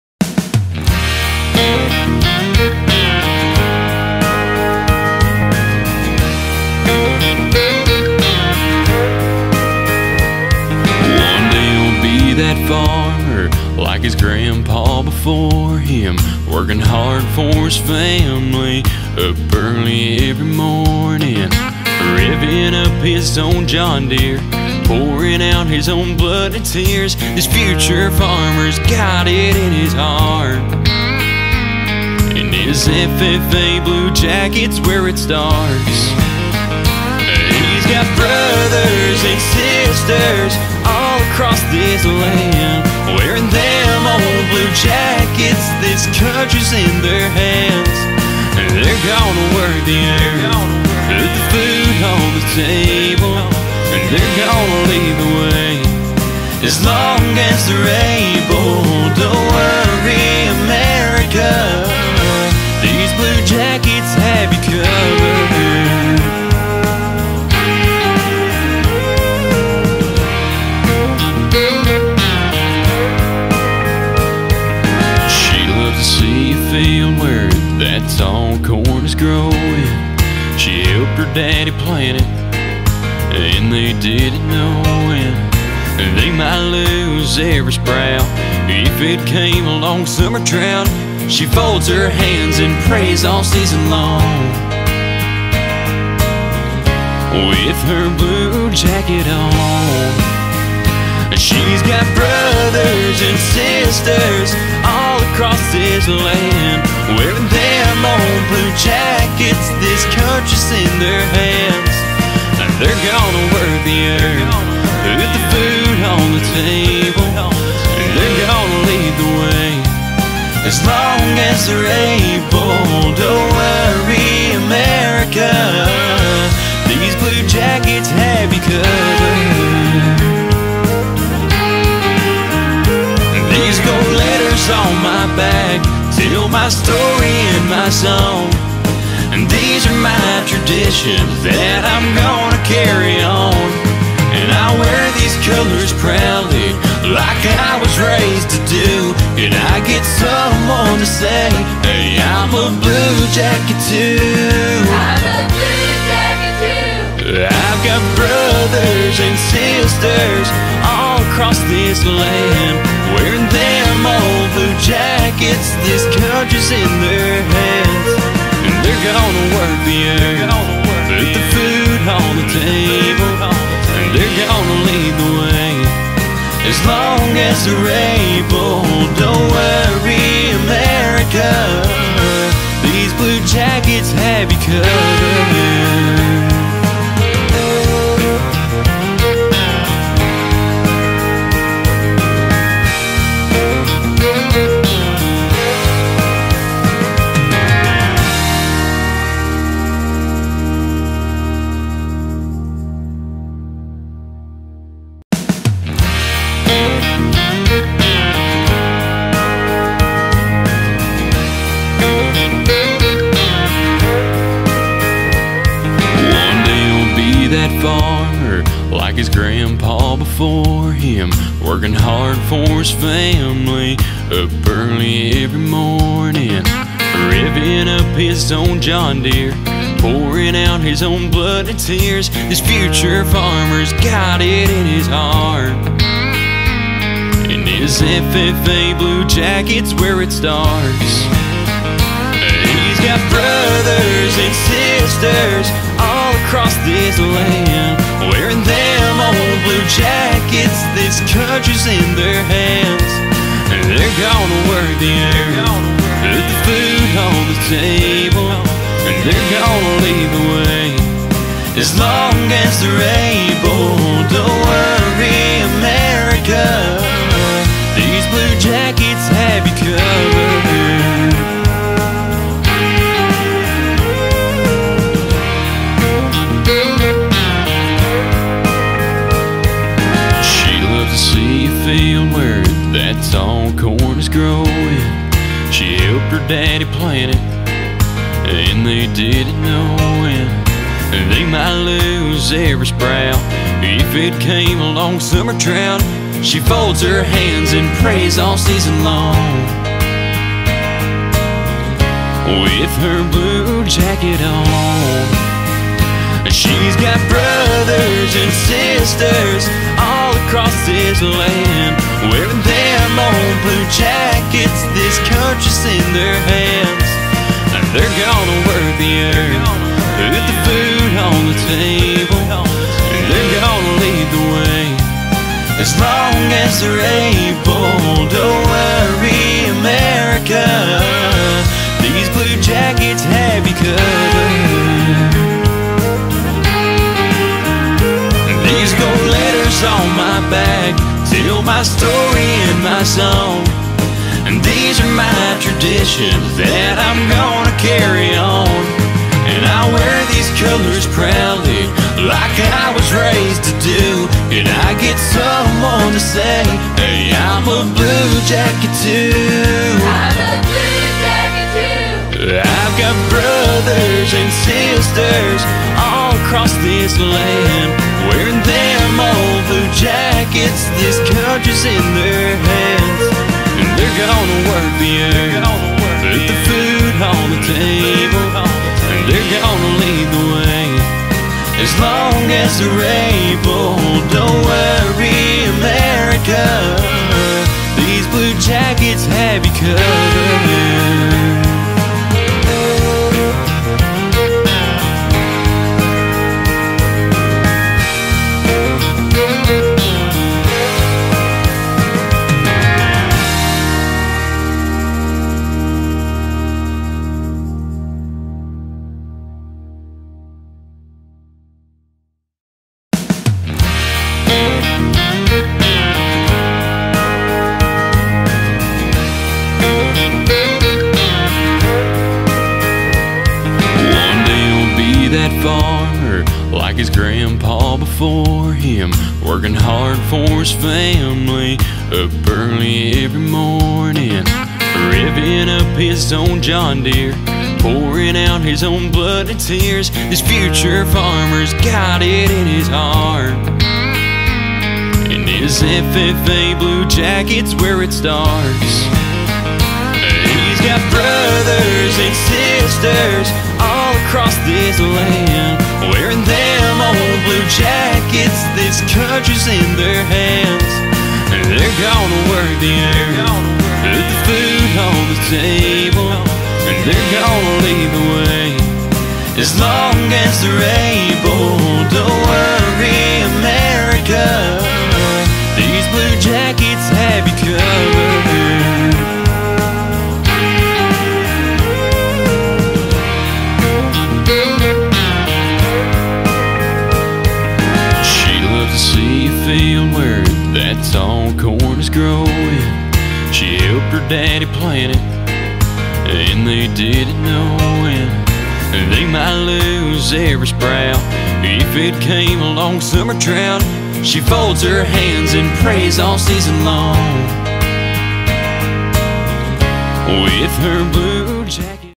Hinsdale FFA 2020 Chapter Awards
Since the Hinsdale FFA wan't able to have an in-person banquet this year, here is a slideshow on all they accomplished this year.